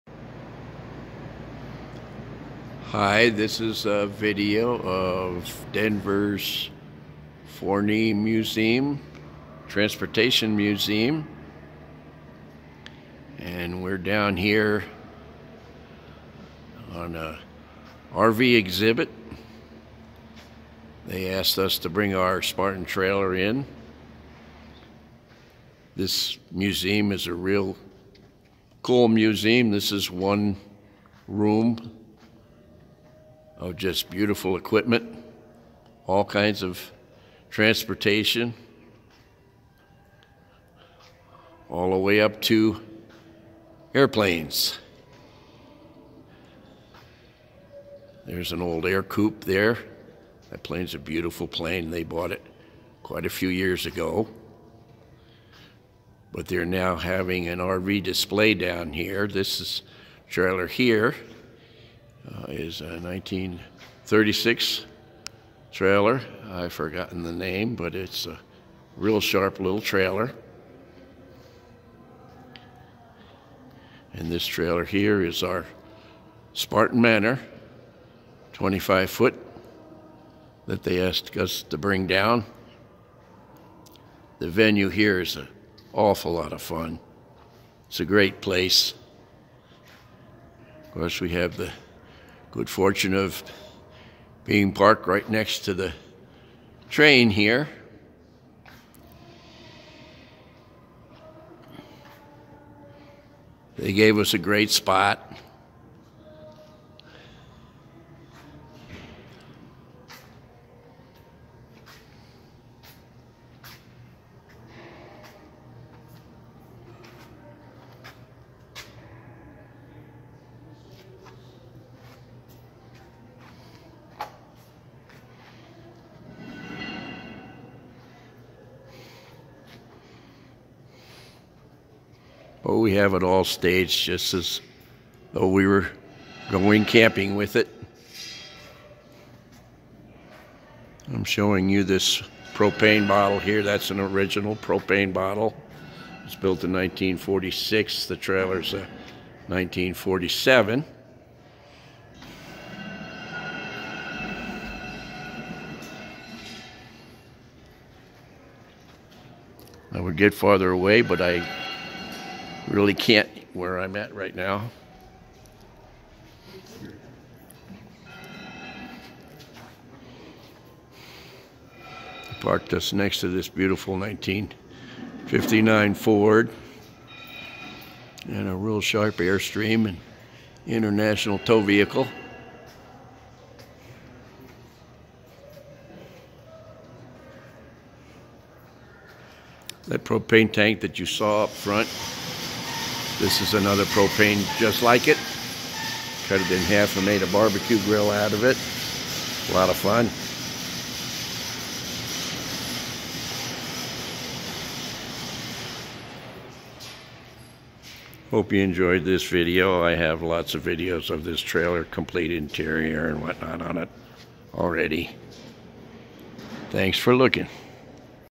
1947 SPARTAN MANOR IN DENVER'S FORNEY MUSEUM
BEING OFFERED AT RUSSO ANS STEELE IN SCOTTSDALE ON JAN 17TH